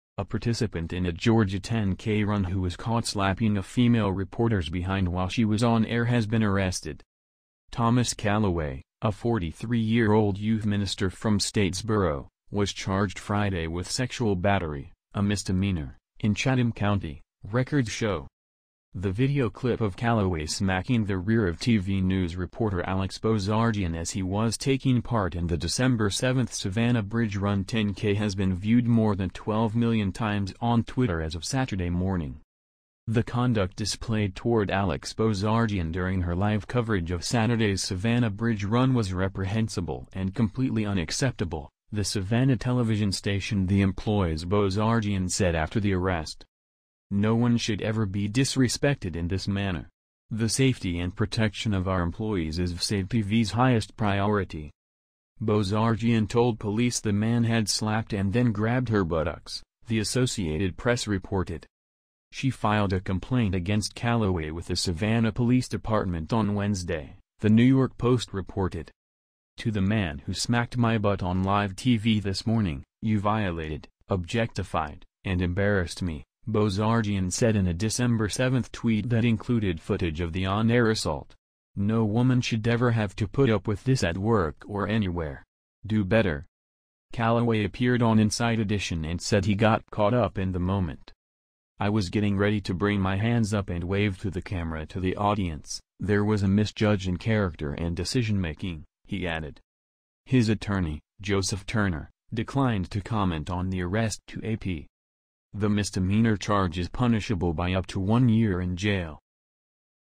Georgia youth minister Thomas Callaway, 43, arrested for slapping reporter's rear on live TV
A participant in a Georgia 10K run who was caught slapping a female reporter's behind while she was on air has been arrested.

Thomas Callaway, a 43-year-old youth minister from  Statesboro, was charged Friday with sexual battery, a misdemeanor, in Chatham County, records show.

The video clip of Callaway smacking the rear of TV news reporter Alex Bozarjian as he was taking part in the Dec. 7 Savannah Bridge Run 10K has been viewed more than 12 million times on Twitter as of Saturday morning.

“The conduct displayed toward Alex Bozarjian during her live coverage of Saturday’s Savannah Bridge Run was reprehensible and completely unacceptable,” the Savannah television station the employs Bozarjian said after the arrest.

“No one should ever be disrespected in this manner. The safety and protection of our employees is WSAV-TV’s highest priority.”

Bozarjian told police the man had slapped and then grabbed her buttocks, the Associated Press reported.